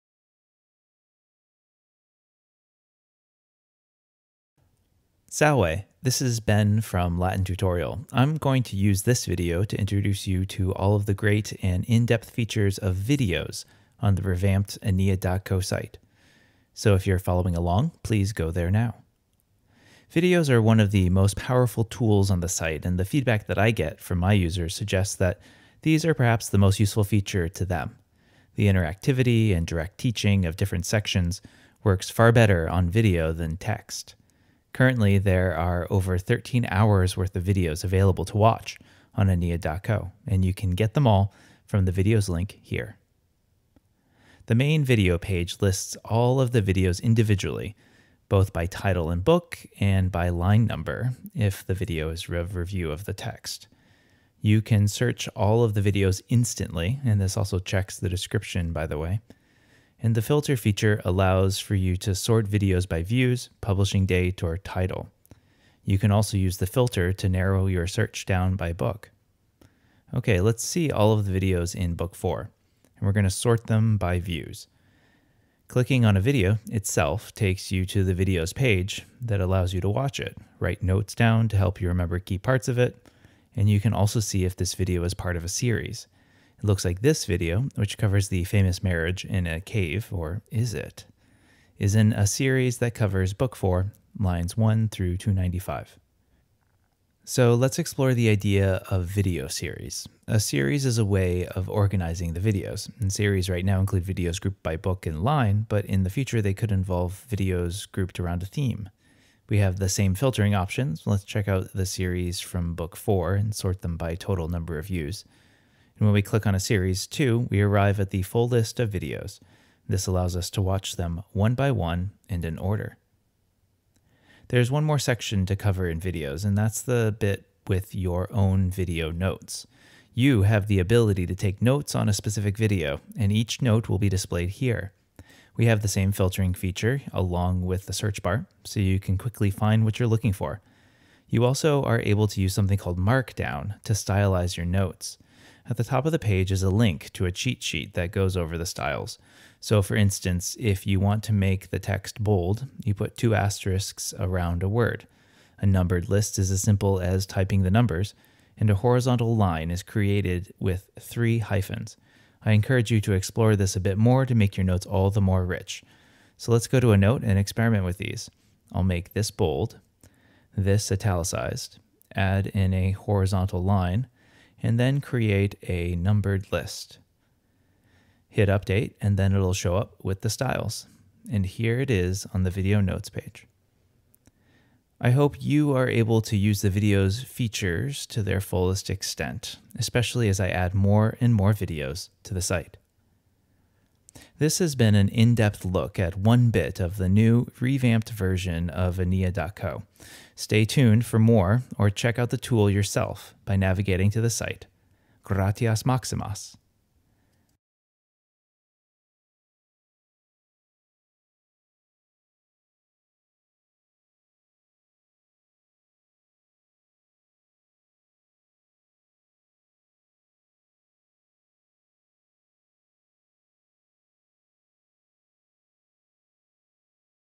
Aeneid.co: Videos in Depth
This video covers the video section on the revamped version of aeneid.co. If you're interested in reading Vergil's Aeneid in Latin, check this site out, and use these in depth videos to help you learn about all of the cool features this site has to offer!